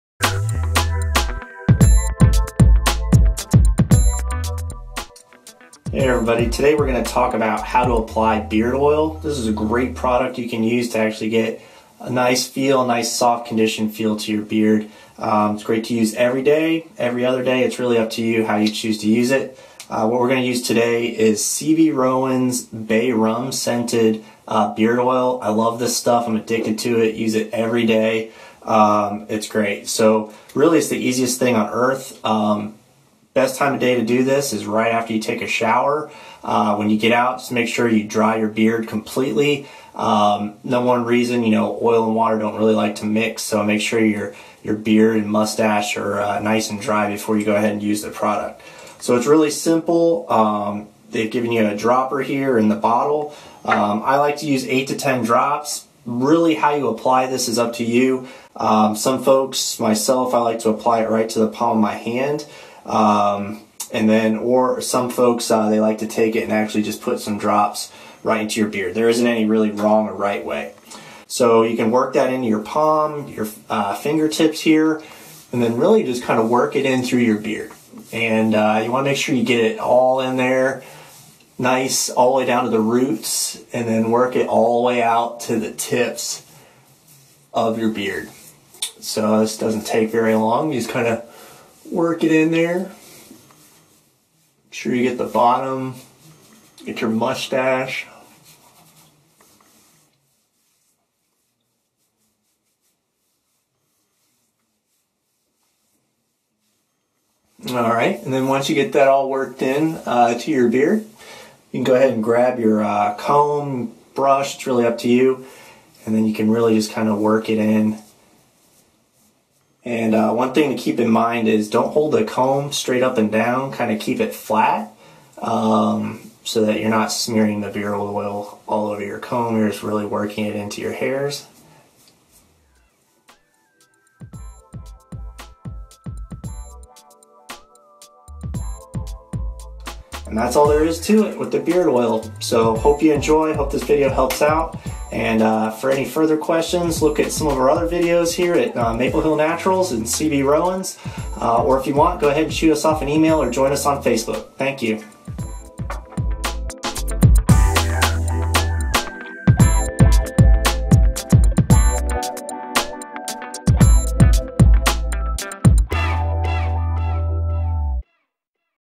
C.B. Rowan's Mens Care: How to Apply Beard Oil
Curious how to apply beard oil? Watch C.B. Rowan himself apply beard oil for hair and skin conditioning.

This all natural beard oil uses a premium blend of natural oils along with essential oils to give you the softest beard while conditioning your face at the same time. It moisturizes, strengthens, and promotes the growth of your beard while preventing split ends. All our products are hand crafted in the USA using the finest natural ingredients. Included dropper makes applying beard oil a snap.